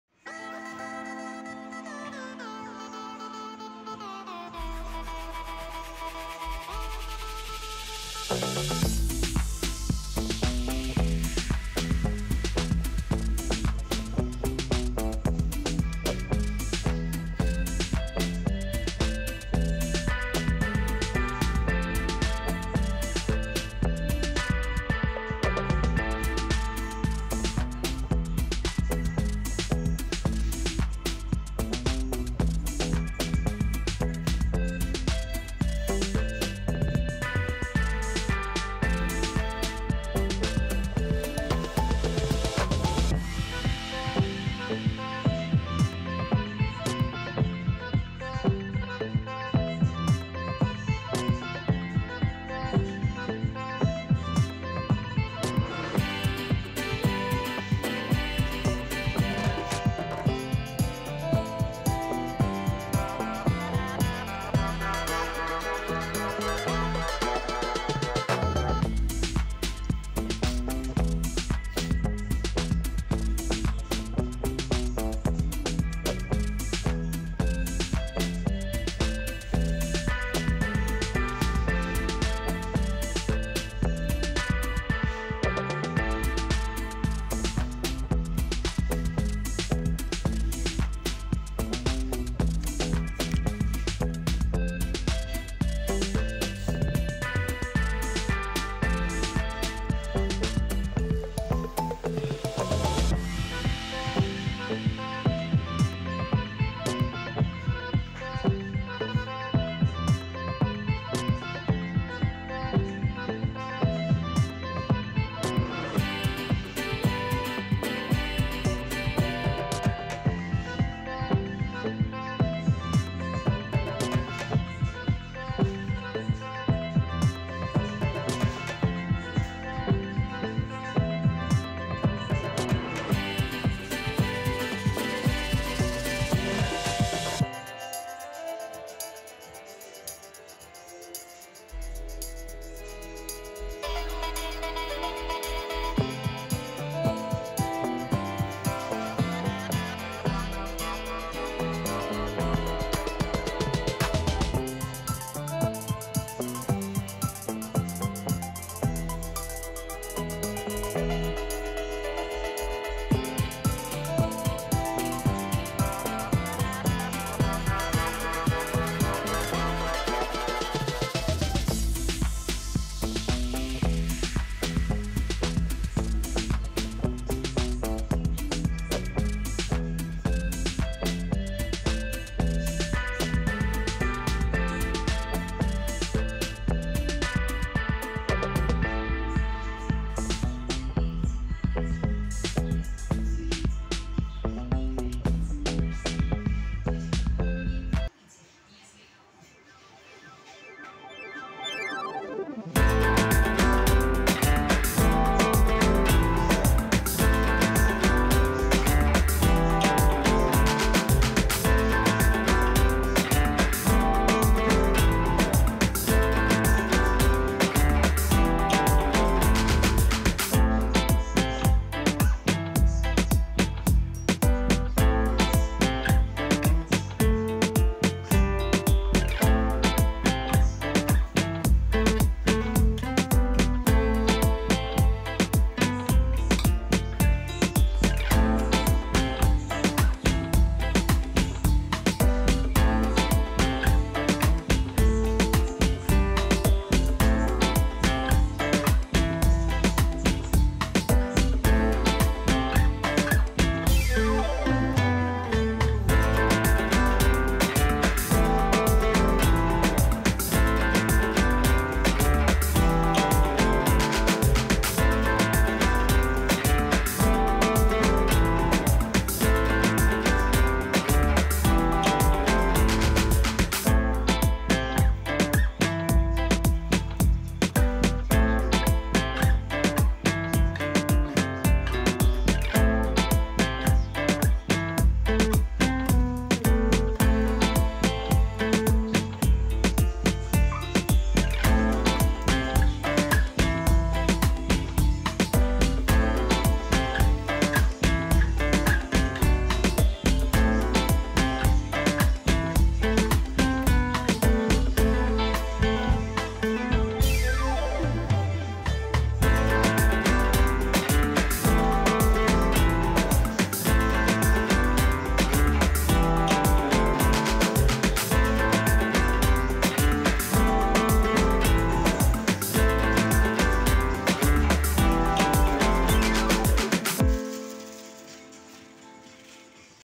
Пляжные шлёпанцы в магазине Kari😉
Магазин Кари Новокузнецк, обзор пляжной обуви 😁